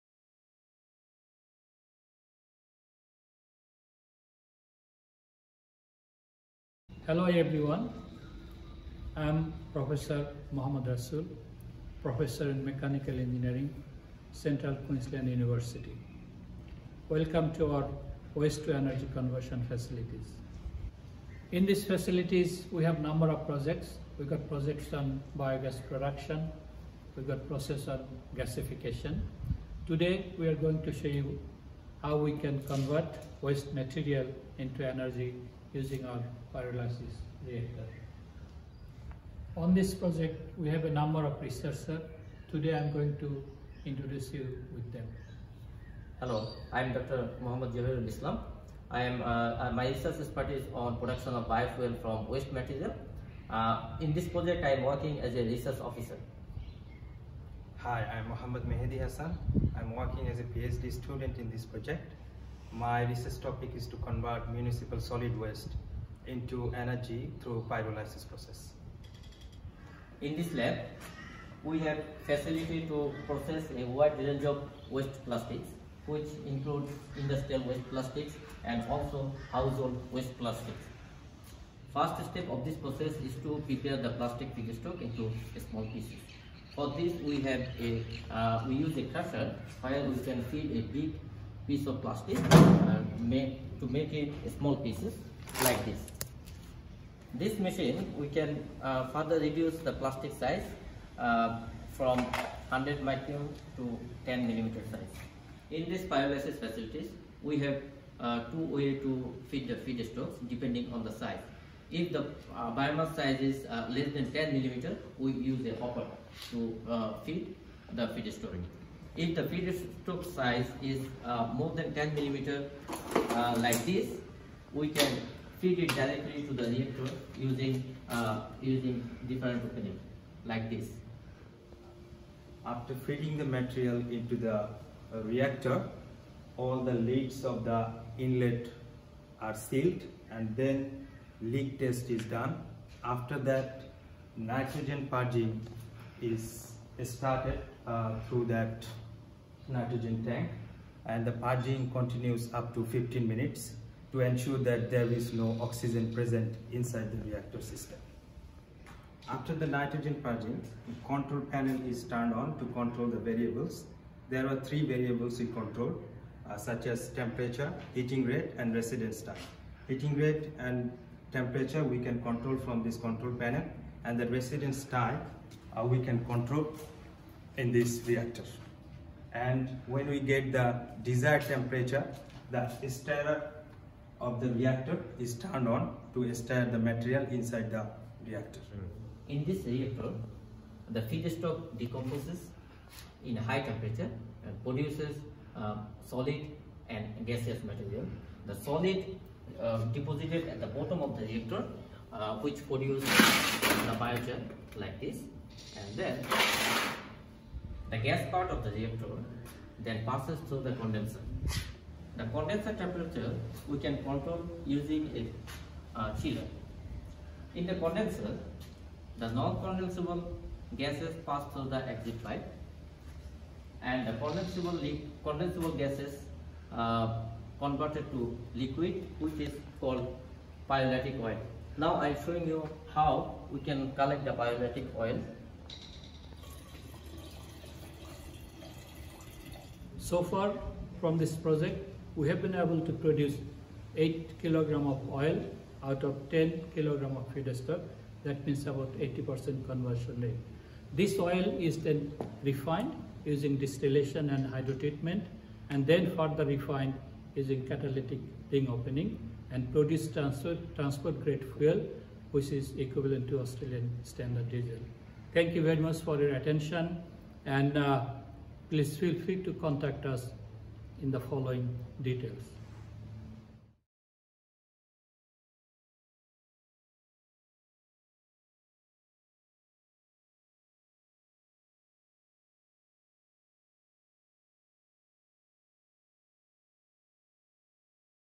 Converting Waste Plastics to Australian Standard Diesel
Professor Mohammad Rasul and his team introduce us to their Waste to Energy Conversion Facility at Central Queensland University, Australia, and talk us through their process for converting municipal (landfill) waste plastics to Australian standard diesel.

A special thanks goes to Waseef Arshad for his videography and production.